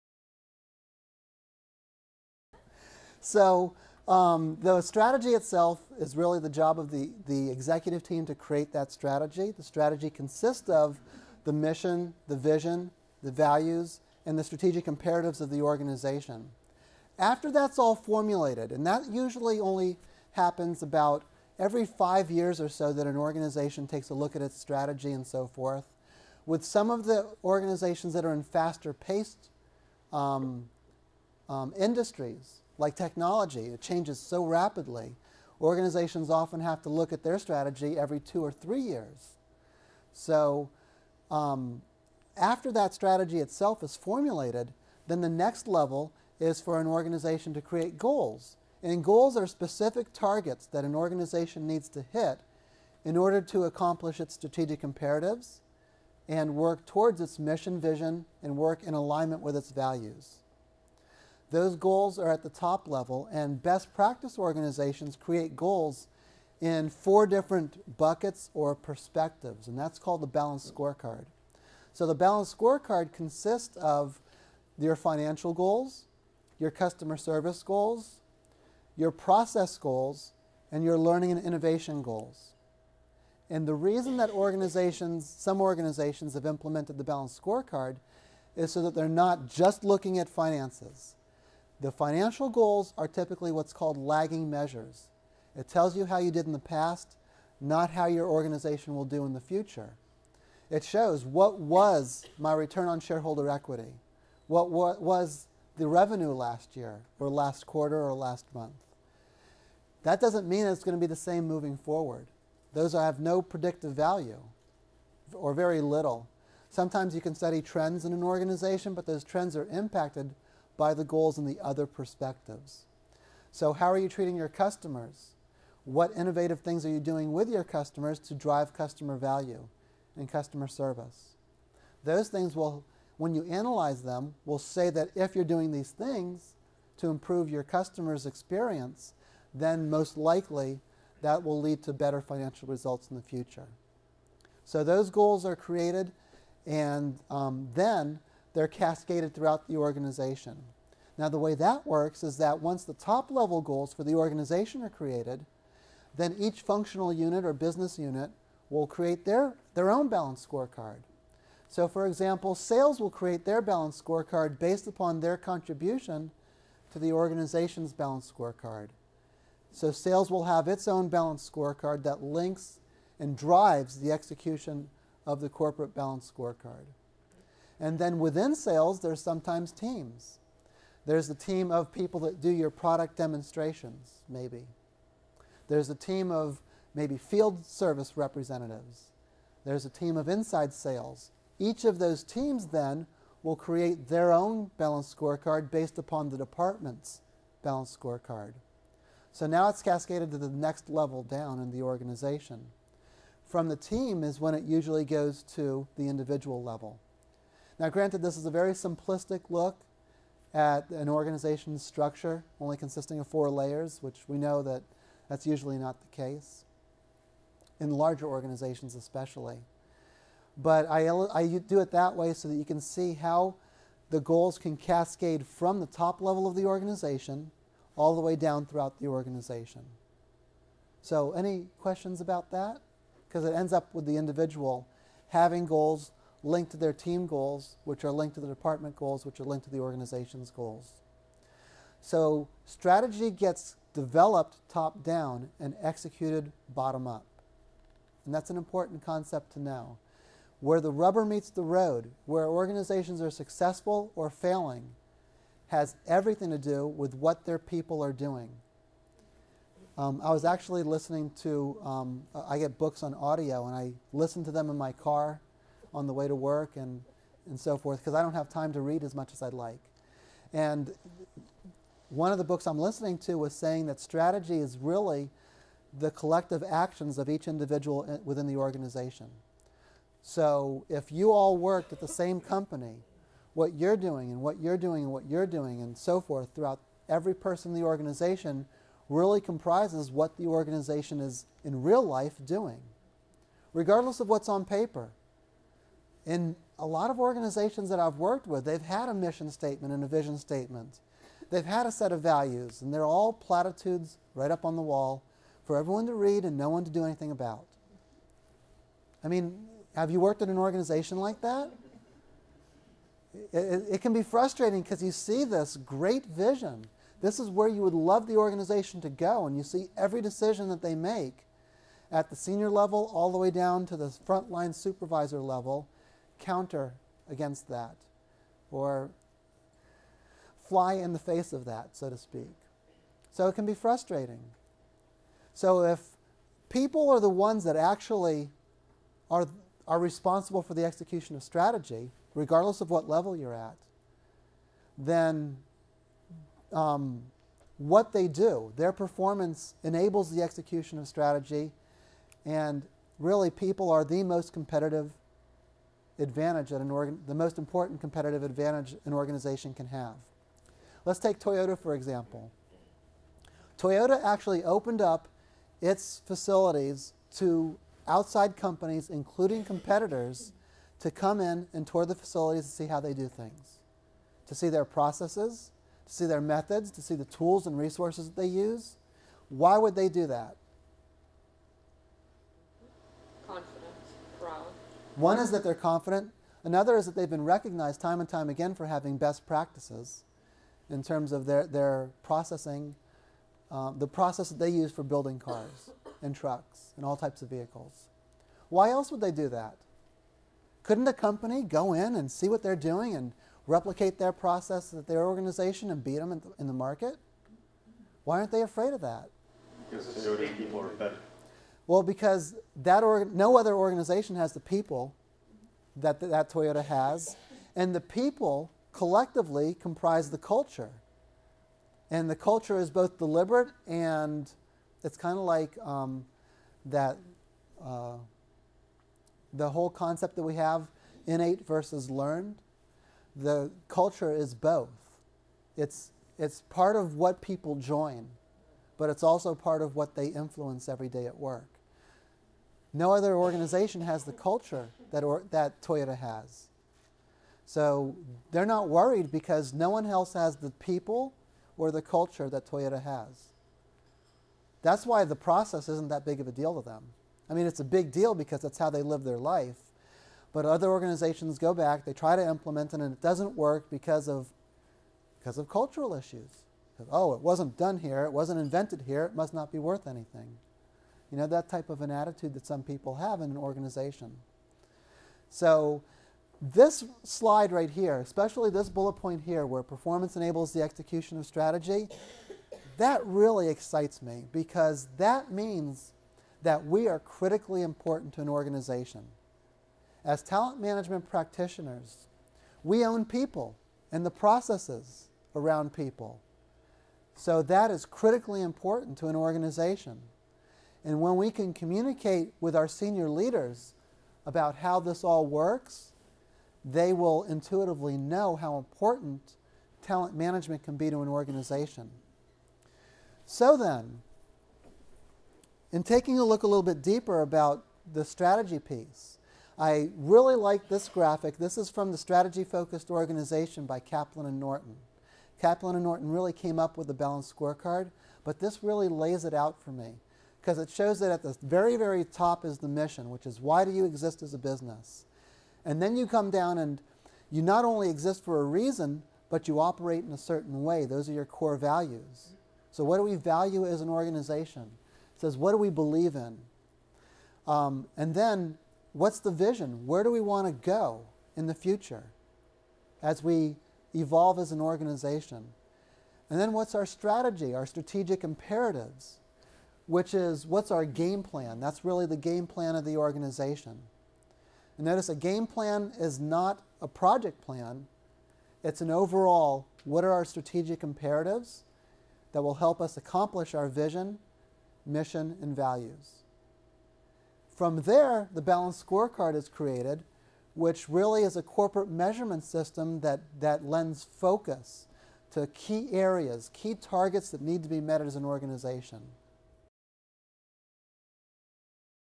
Aligning Training, HR, and Strategy pt 5
The Learn.com Tour on Talent Presentation by Michael Sabbag.